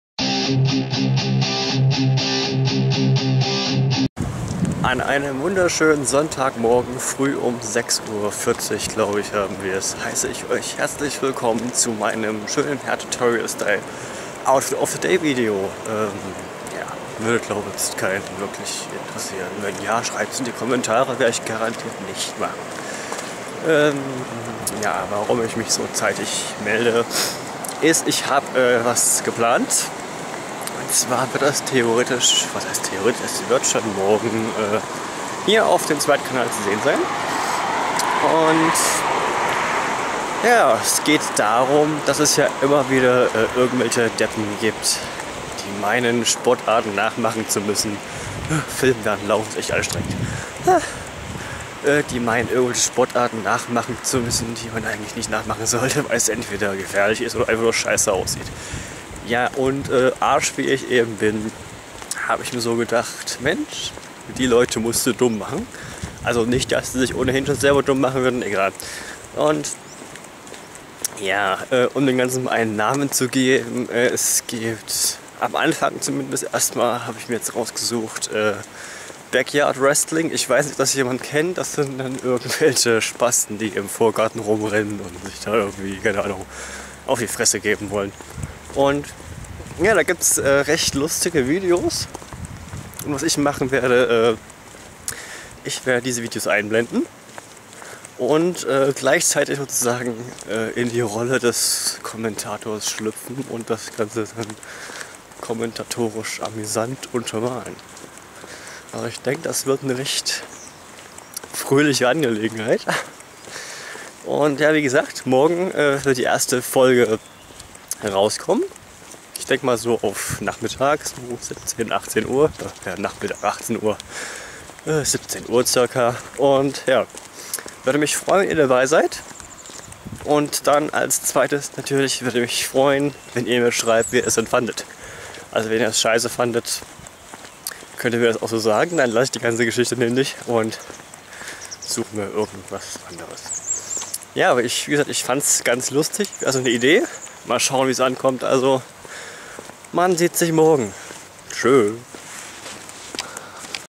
AnkündigungsVlog ( Dumme Menschen, müssen dumm gemacht werden ! )
Kleines Ankündigungsvideo !!! MORGEN schon die ERSTE Folge !!!
zu einem neuen Konzept, bei dem ich mich über dumme Menschen lustig mache , welche sprtarten total mies nachmachen :)